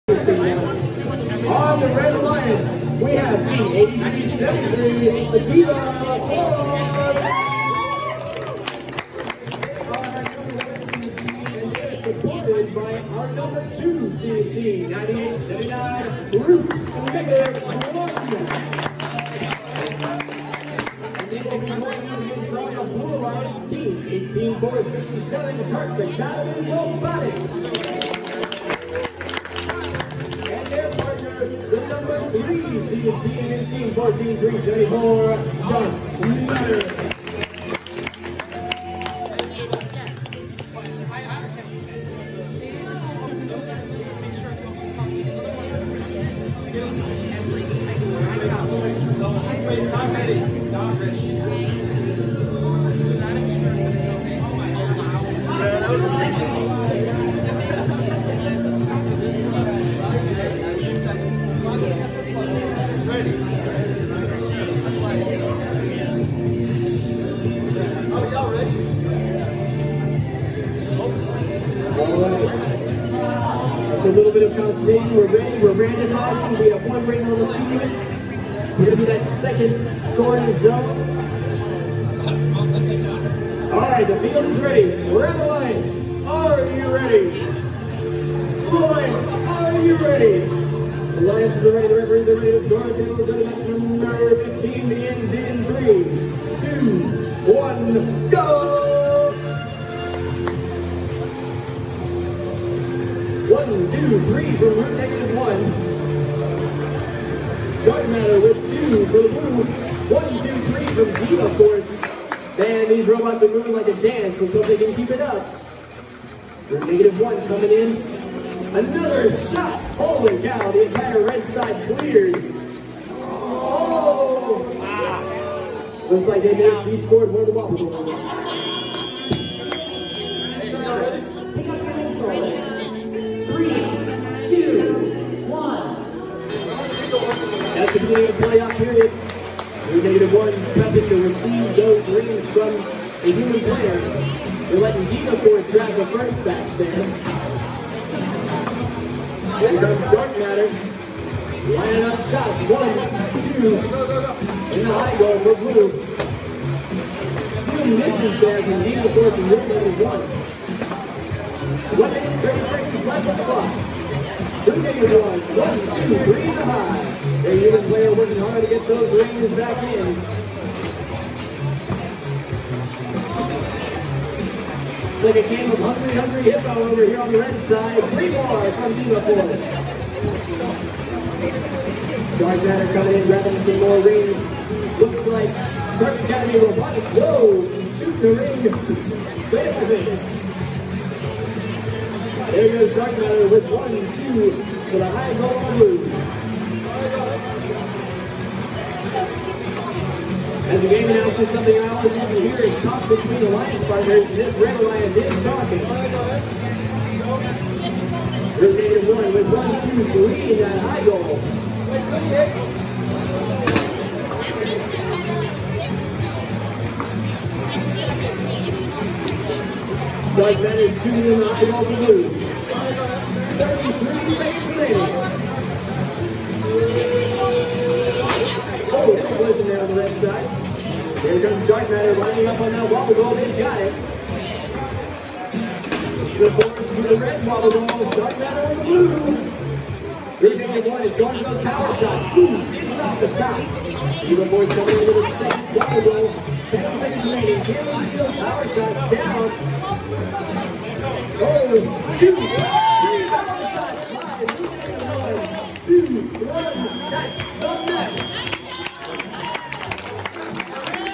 2020-2021 Ultimate Goal Ultimate Rematch (traditional) Qualification Match 15
Blue Alliance (14374, 18467): 128 points
Red Alliance (8373, 9879): 388 points